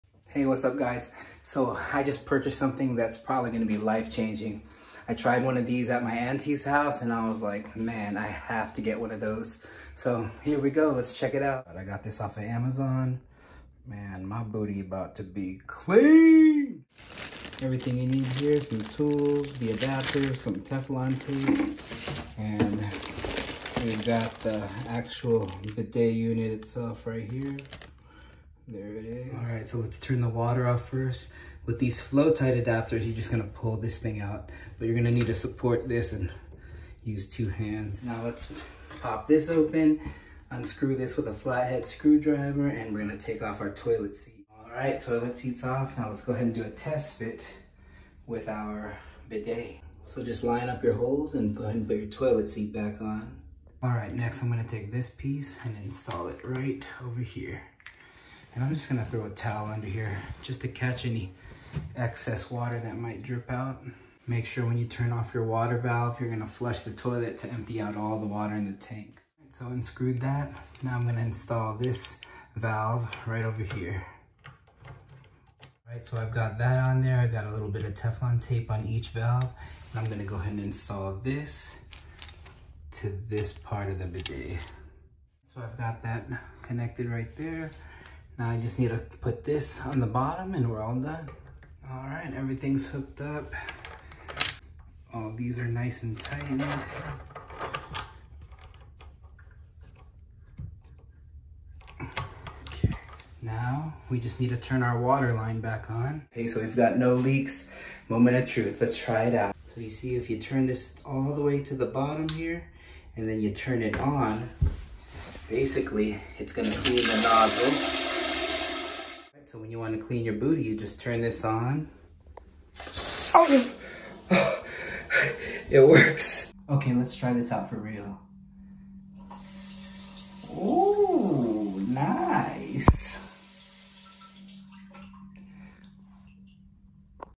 HOW TO INSTALL LUXE BIDET NEO 120. UNBOXING, INSTALLATION, & FIRST TRY REACTION
Luxe Bidet Neo 120

Life changing purchase! Tried out a bidet at someone's house and had to buy one for my household. Very easy to install... only took a few minutes.  Get yourself so fresh and so clean AND save on some toilet paper!

Note: Need a toilet seat with a flat bottom, otherwise you can purchase some bumpers for the bottom of your existing seat